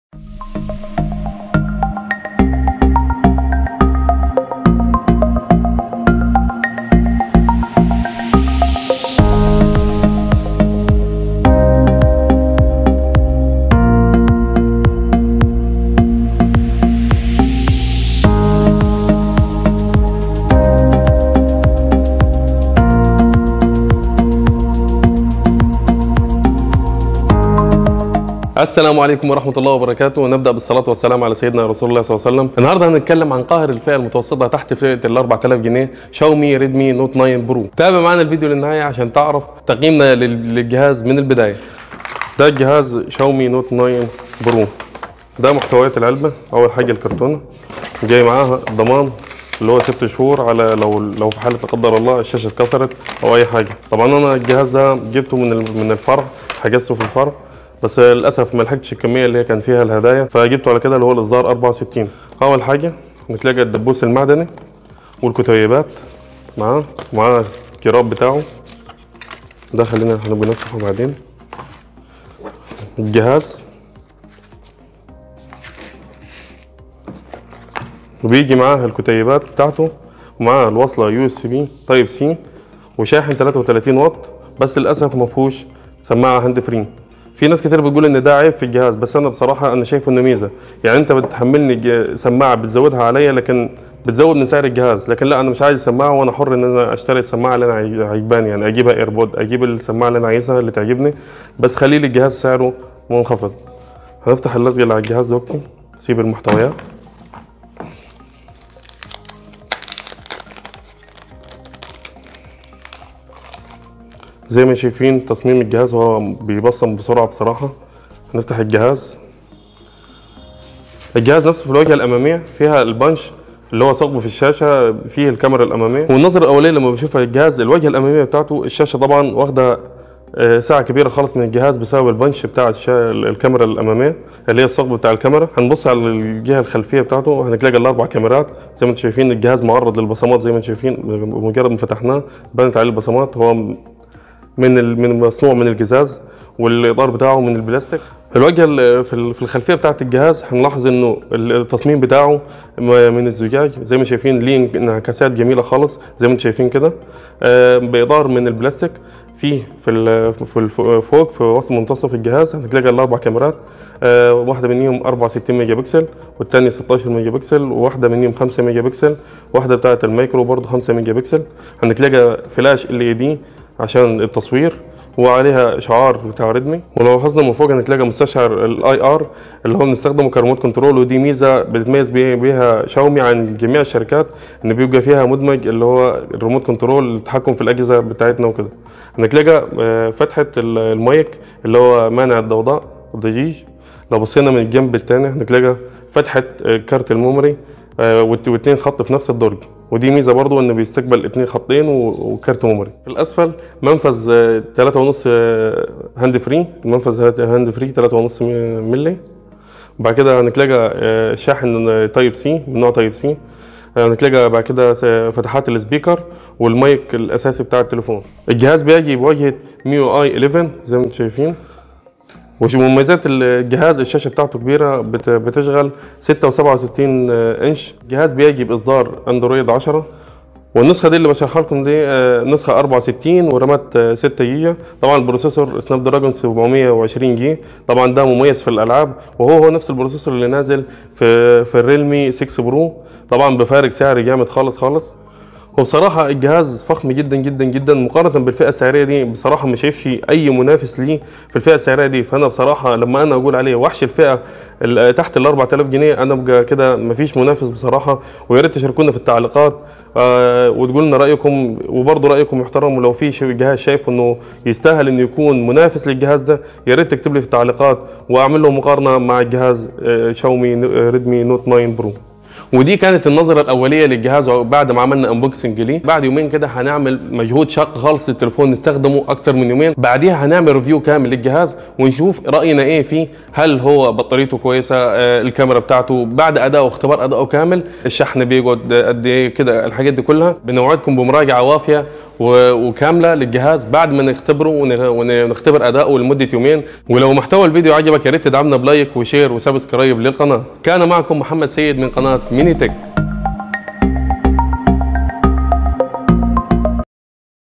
فتح صندوق ونظره أوليه شاومي ريدمي نوت 9 برو || xiaomi redmi note 9 pro
موبيل شاومى الاحدث ريدمى نوت 9 برو Redmi note 9 pro هو افضل موبيل من شاومى وحش الفئه المتوسطة وبافضل سعر لحد دلوقتى .